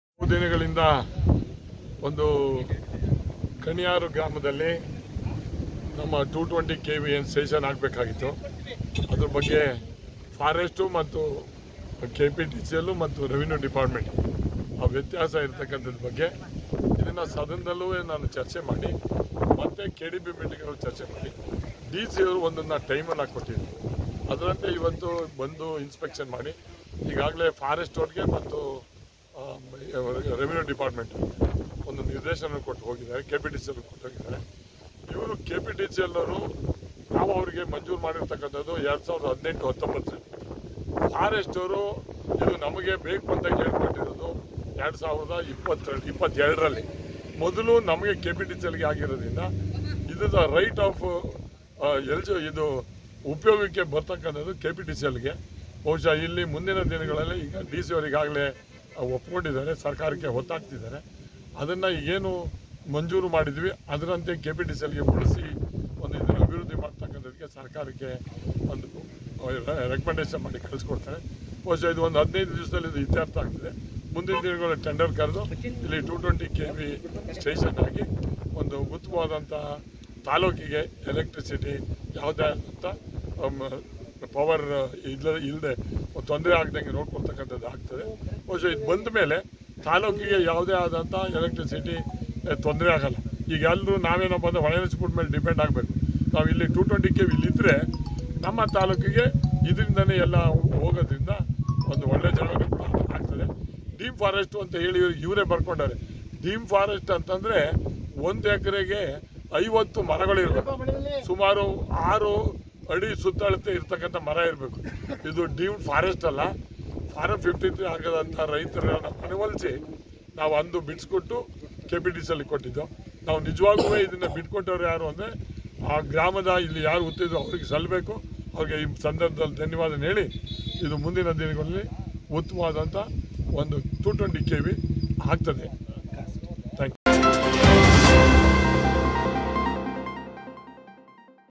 220 KV ವಿದ್ಯುತ್ ಸ್ಥಾವರ ವಿಚಾರವಾಗಿ ಹಾಸನ ಡಿಸಿ ಮತ್ತು ಅಧಿಕಾರಿಗಳೊಂದಿಗೆ ಸ್ಥಳ ಪರಿಶೀಲನೆ ನಡೆಸಿದ ಶಾಸಕ ಎ ಮಂಜು
220 ಕೆವಿ ವಿದ್ಯುತ್ ಸ್ಥಾವರ ವಿಚಾರವಾಗಿ ಹಾಸನ ಡಿಸಿ ಮತ್ತು ಅಧಿಕಾರಿಗಳೊಂದಿಗೆ ಸ್ಥಳ ಪರಿಶೀಲನೆ ನಡೆಸಿದ ಶಾಸಕ ಎ ಮಂಜುhassan